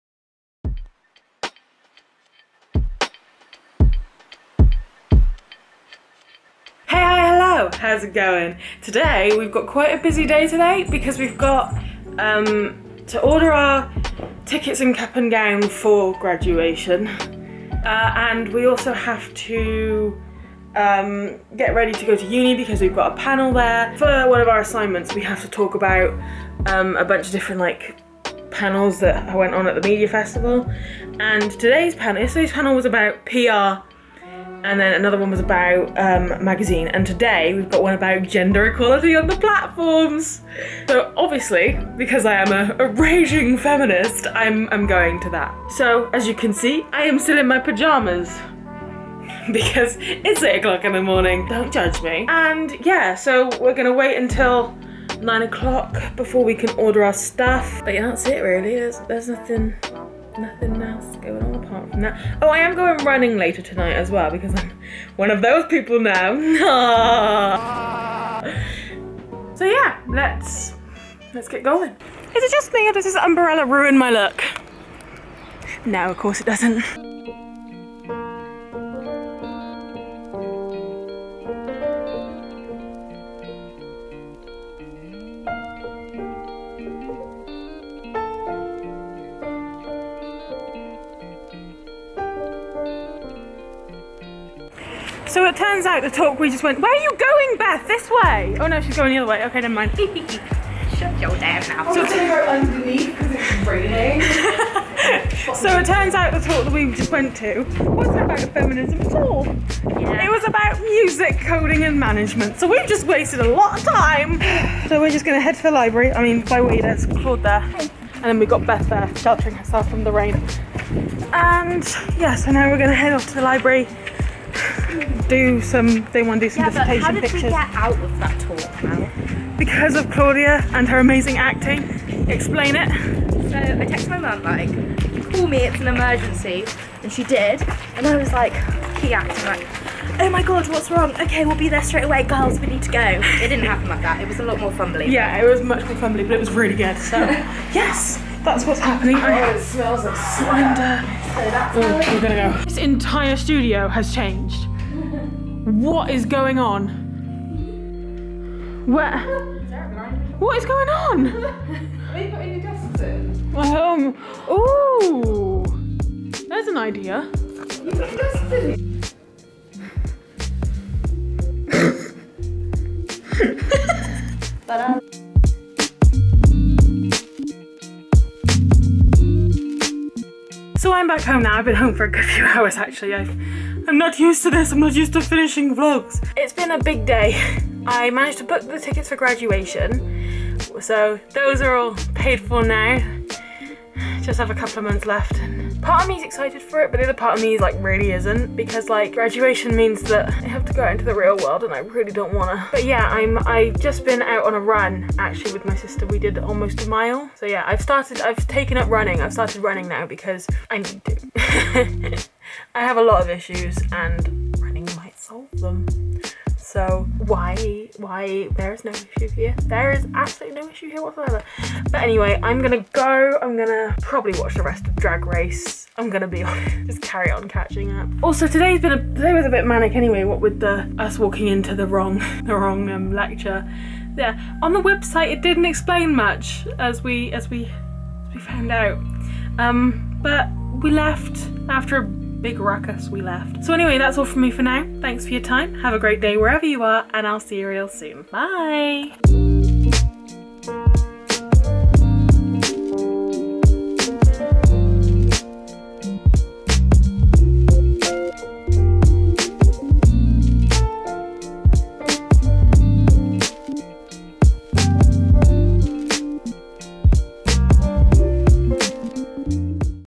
WE WALKED INTO THE WRONG LECTURE | Vloggy Vlog 45
With not so much of a heavy heart, I've decided to postpone/cancel Monthly Madness for a second time as, once again, it was just turning into many days of me just solely talking. I feel daily vlogging once in a while will just, ultimately, interest me more and make the content much better for you to watch. So, this is the first of many "odd-job" vlogs that will be popping up hither and thither. What a way to kick off the new format, though! A total goof on our part. I hope you enjoy this anyway!